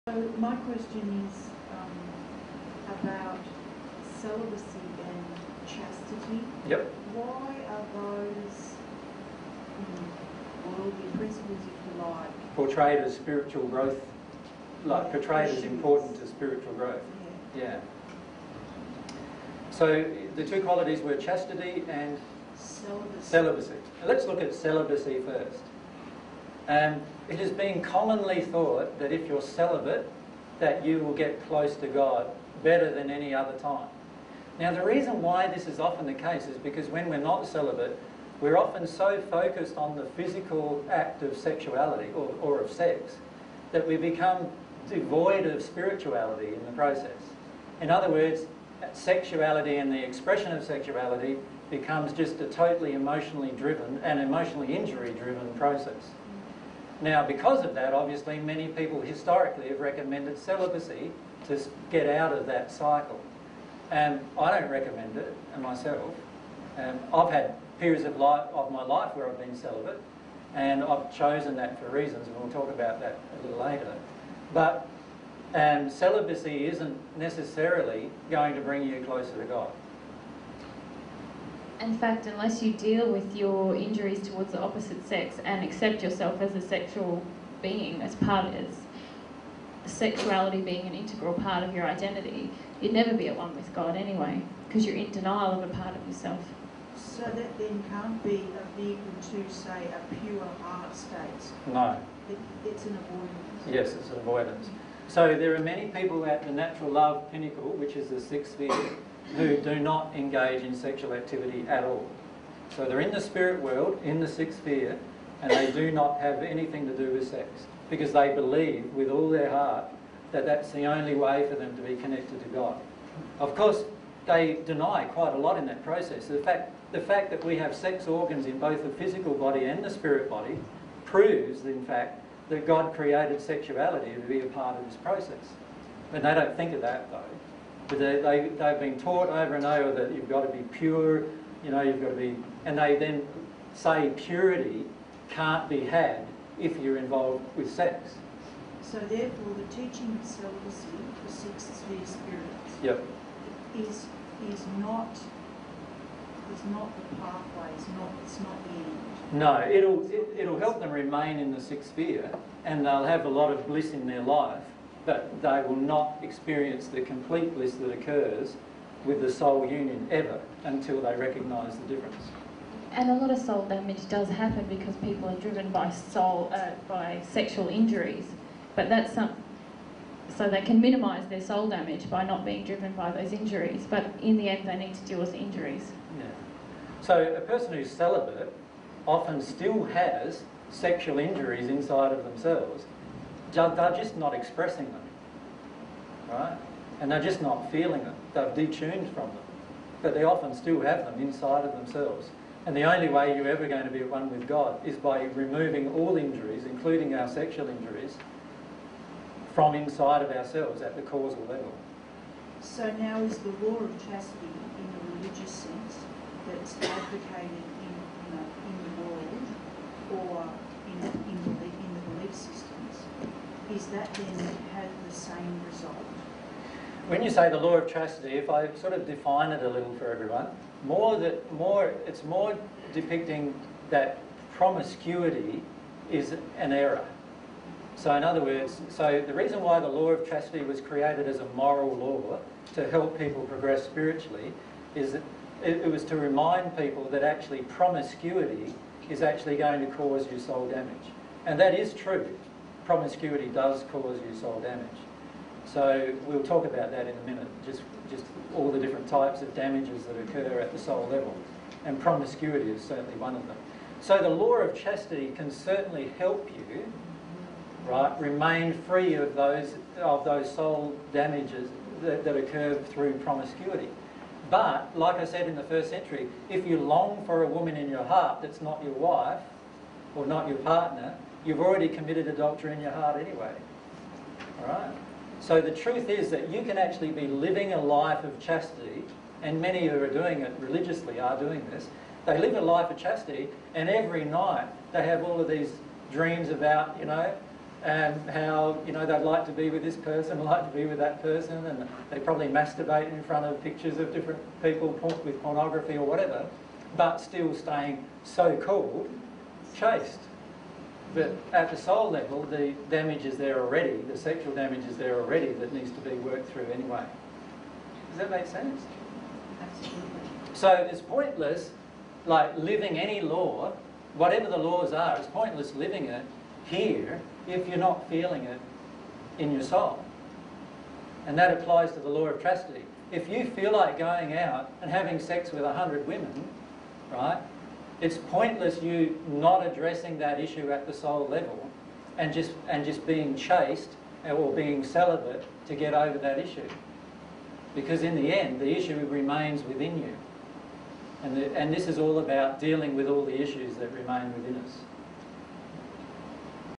Jesus on Celibacy and Chastity
Jesus discusses celibacy and chastity.

This is a section of the presentation by Jesus and Mary entitled "20090314 Human Relationships - Sex & Sexuality S1P1".

dtstart:003835:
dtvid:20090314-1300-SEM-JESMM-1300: